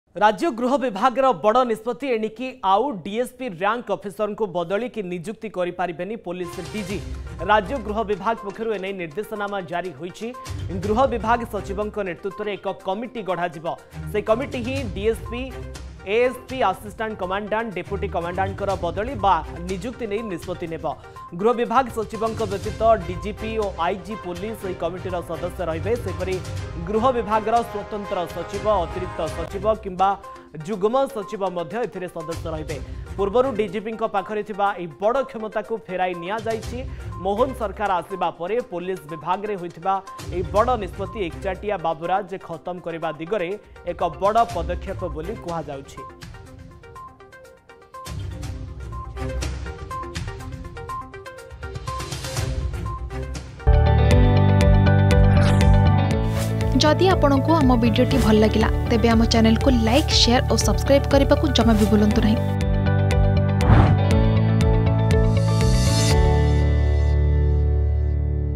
ଡିଏସପି ର୍ୟାଙ୍କ ଅଫିସରଙ୍କୁ ବଦଳି କି ନିଯୁକ୍ତି ଦେଇପାରିବେନି ଡିଜିପି || DG of Police
ଗୃହ ବିଭାଗର ବଡ଼ ନିଷ୍ପତ୍ତି । ଡିଏସପି ର୍ୟାଙ୍କ ଅଫିସରଙ୍କୁ ବଦଳି କି ନିଯୁକ୍ତି ଦେଇପାରିବେନି ଡିଜିପି । ଗୃହ ବିଭାଗ ସଚିବଙ୍କ ଅଧ୍ୟକ୍ଷତାରେ ଗଢ଼ା ହେବ କମିଟି ।argusnews